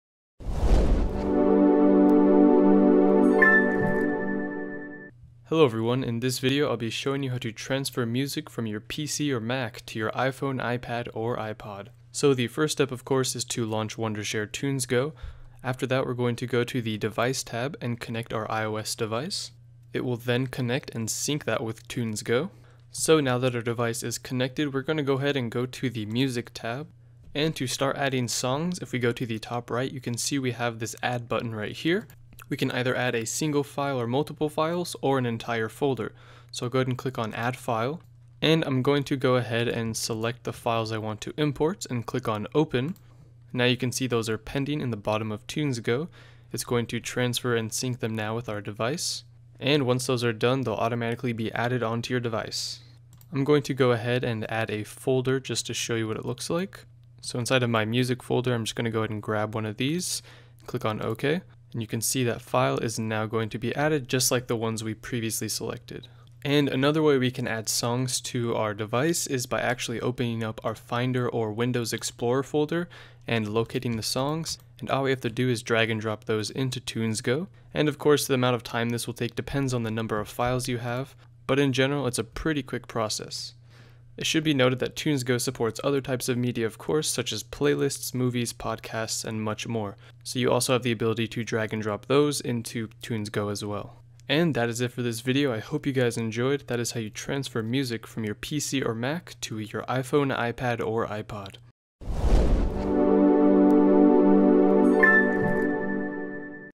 TunesGo: Transfer Music from PC/Mac to IPhone/IPad/IPod
Transfer music without device limitation
1. Transfer your iTunes media files (Music, Playlists, Movie, Podcasts, TV Shows, iTunes U and more) without limits.
a. iPhone/iPod/iPad ← → iTunes.
b. iPhone/iPod/iPad/Android ← → PC/Mac.
c. Android ← → iTunes.
2. TunesGo automatically converts almost any music file to a format supported by your device and iTunes. Compatibility is never an issue.